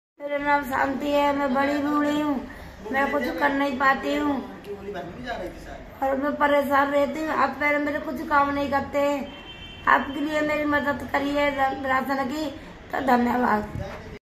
Interviewing the Elderly
This video is part of a series of interactions with the elderly spread across the spectrum. These interactions help us in understanding the issues faced by the elderly. They share their views and experiences which eventually helps Agewell Foundation in advocating for their needs and rights and creating the path for a better life for them.